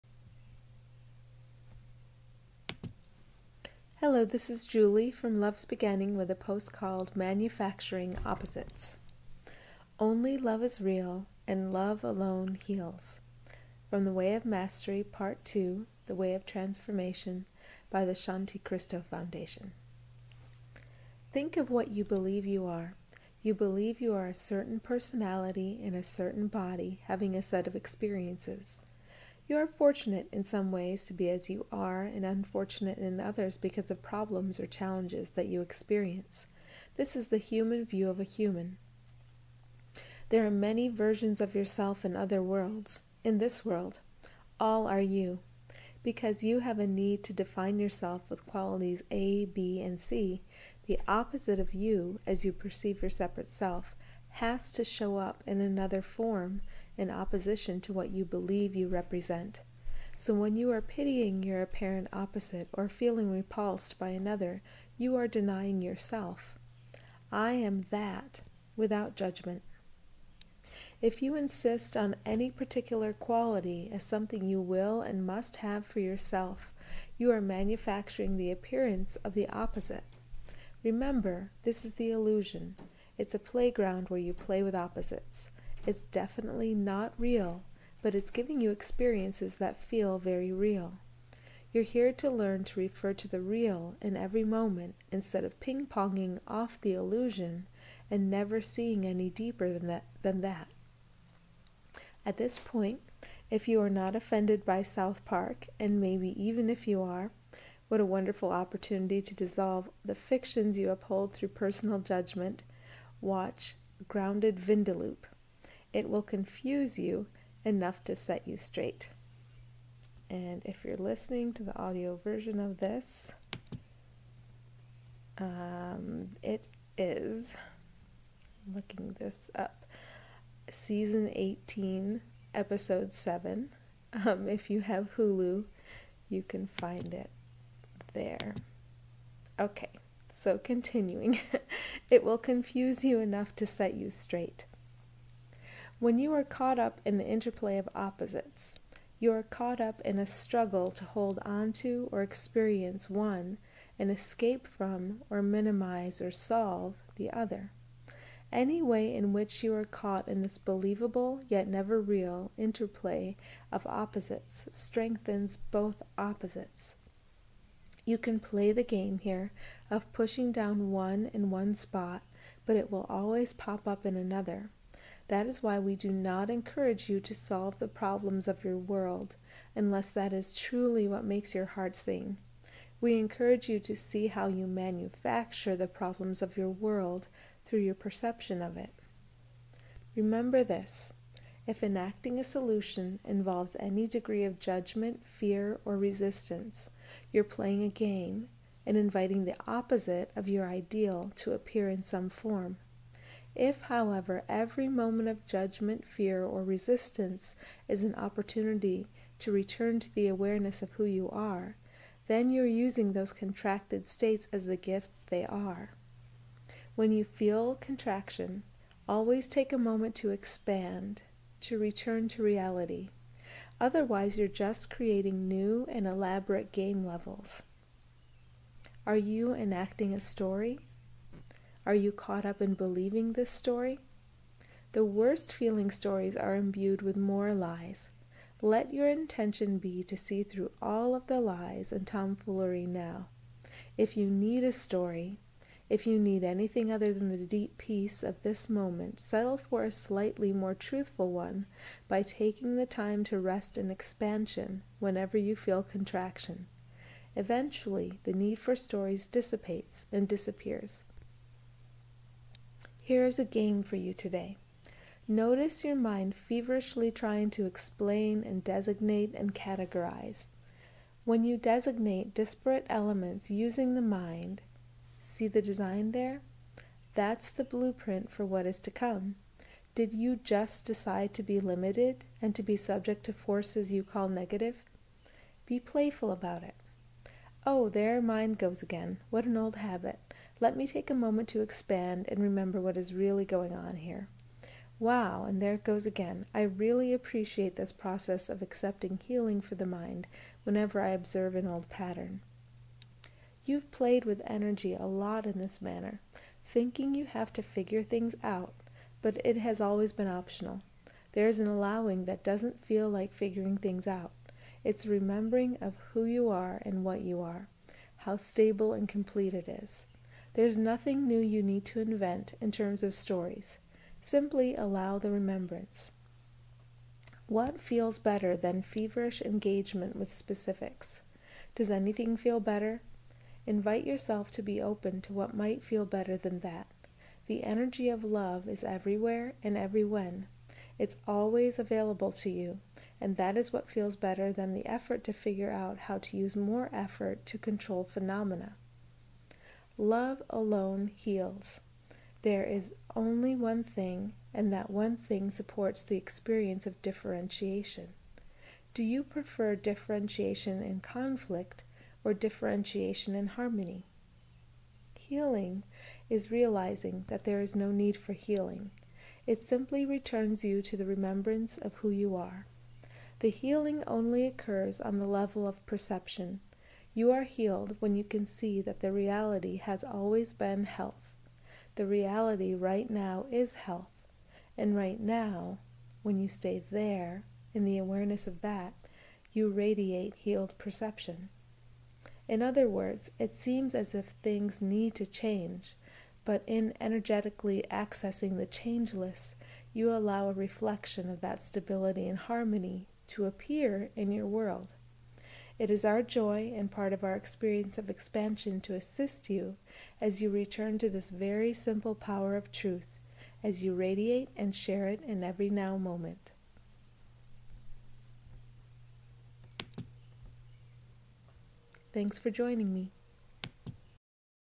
Manufacturing opposites
Because you have a need to define yourself with qualities A, B & C, the opposite of you (as you perceive your separate self) has to show up in another form, in opposition to what you believe you represent.